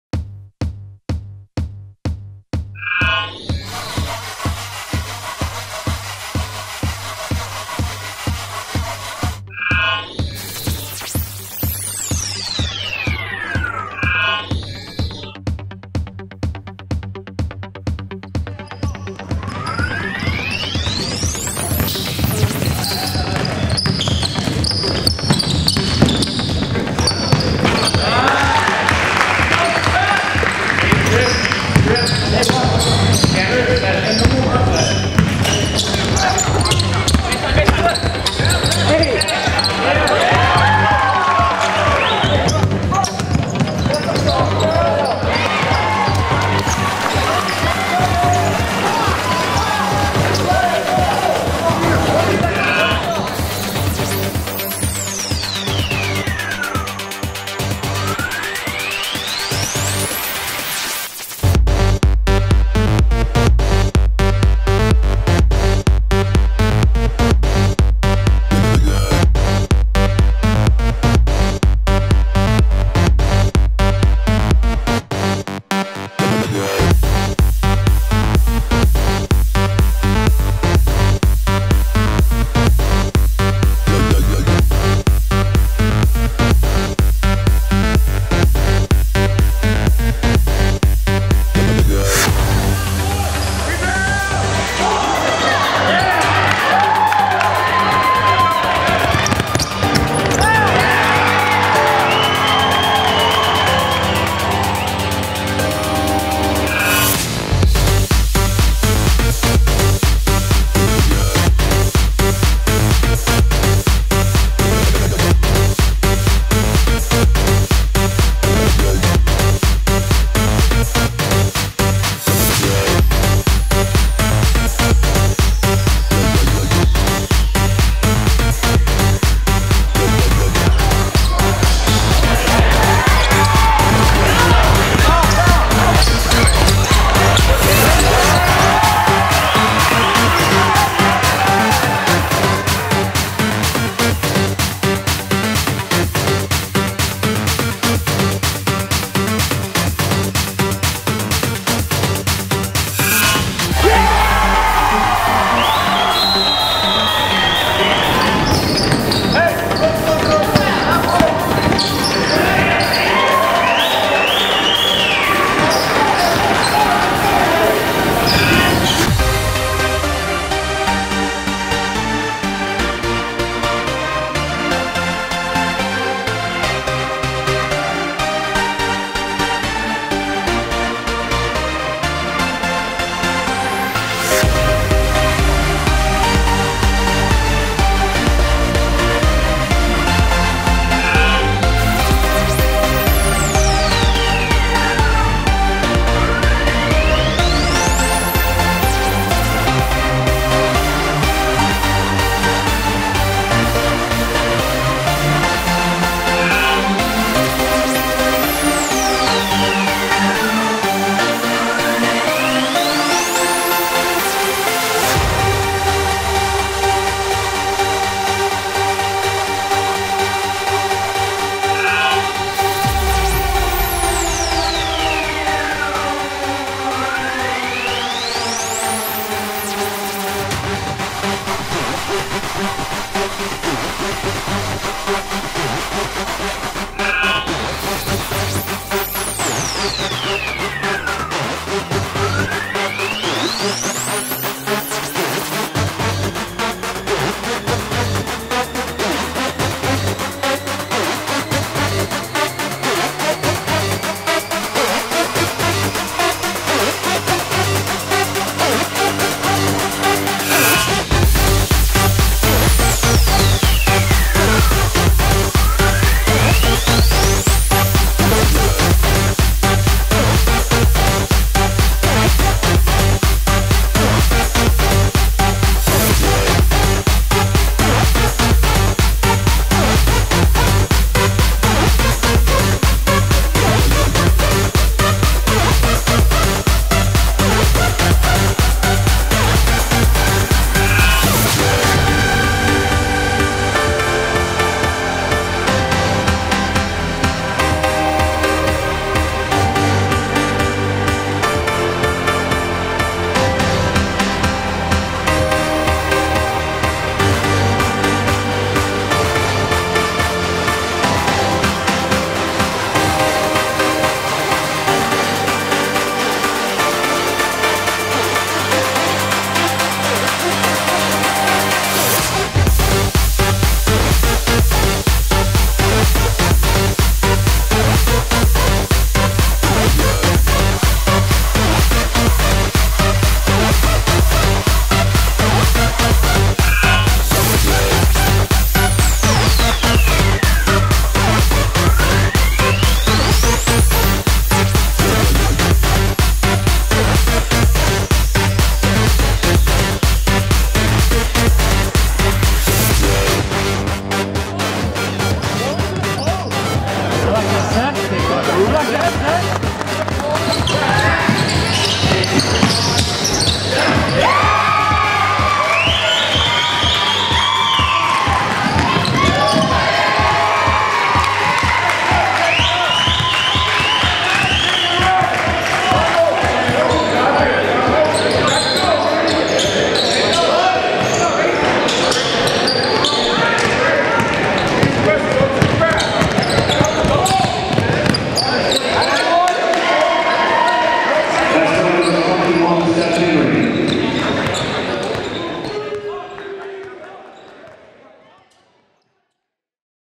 Santa Margarita Eagles PF Seth Henry: 2015-16 Mixtape
Power forward Seth Henry (No. 21, c/o ’18) is a unique basketball player with wide-ranging talents. Blessed with great size and a large wingspan, Henry is able to compete in the post, especially off the dribble drive, which is his most effective scoring option. But Henry is a sharp-shooter as well, arguably one of the best on the team, especially from beyond the arc.

Henry was a fundamental piece and starter for a squad that racked up four consecutive tournament championships on the way to claiming another Trinity League crown.

Henry and his teammates have now collected two Trinity League championships in two seasons.

Two years that included a total of one loss.

Mixtape by -B. Petersen.